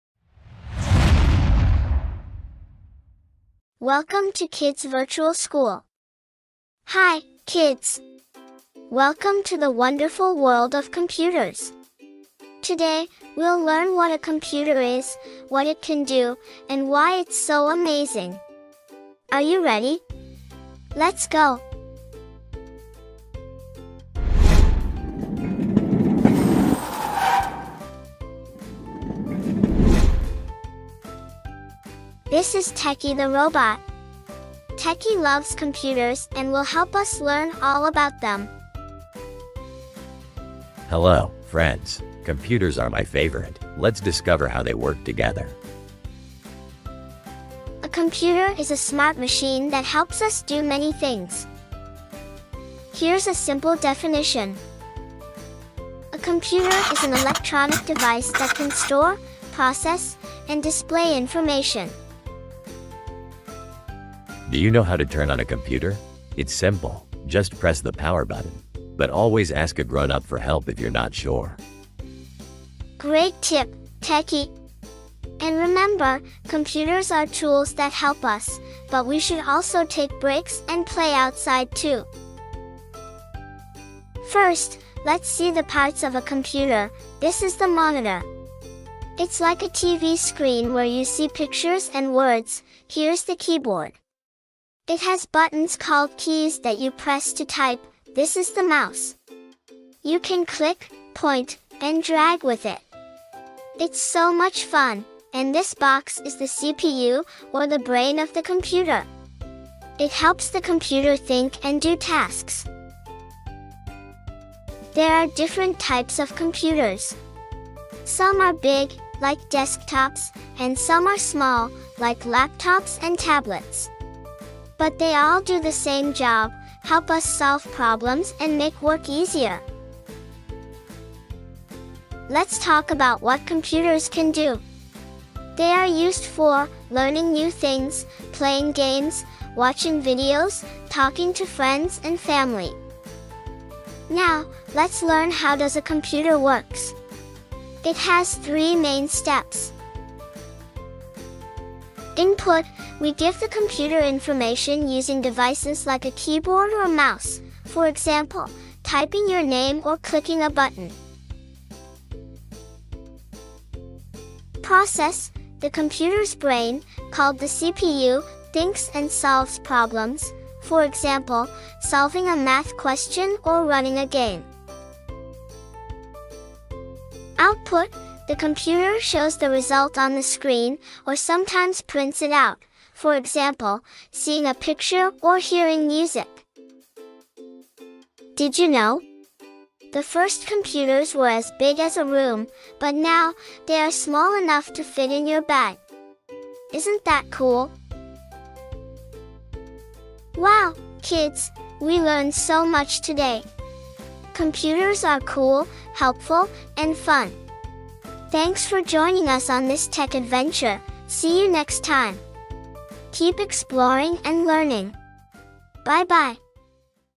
Introduction to Computers for Kids | Learn Basics of Computer Parts and Functions
Discover the exciting world of computers in this fun and engaging video for kids!  Learn what a computer is, its parts like the monitor, keyboard, mouse, and CPU, and how it works step by step. Perfect for beginners, students, and young learners who want to explore technology!